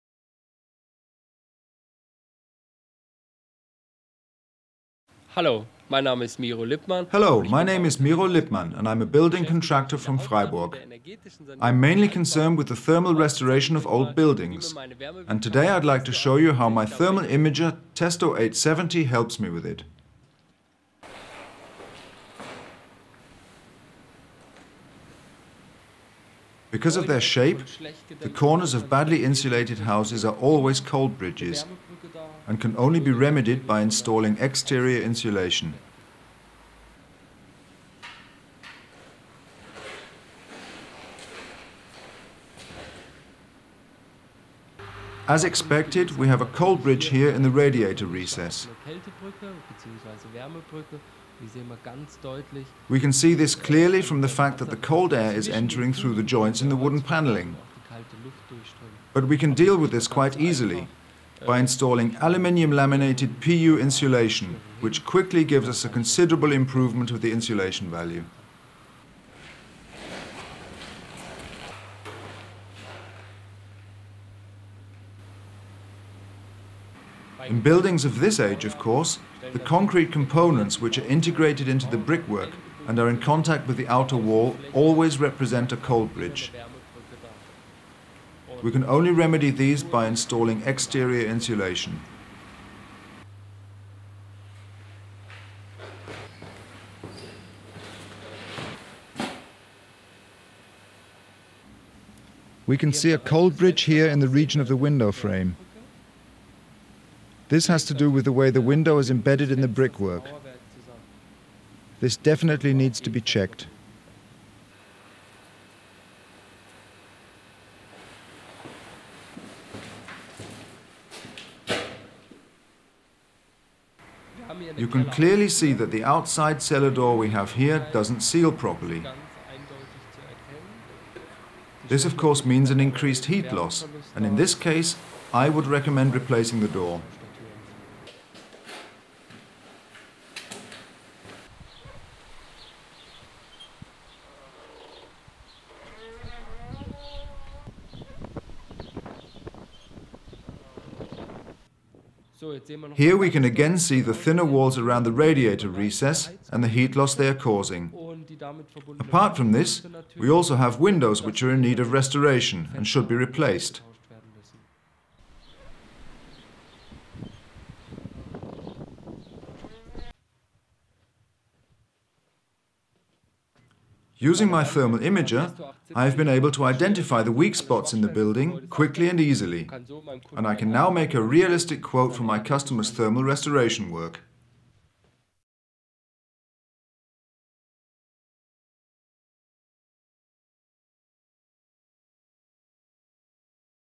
Thermography in the building sector with the thermal imager testo 870 | Be sure. Testo
Find out here how convincing the new thermal imager testo 870 is in the building sector.

testo 870: the thermal imager specially for building contractors.
So you find building faults even more easily.
- High-value detector with 160 x 120 pixels
- Easy operation -- switch on and go
- Pays off after only a few applications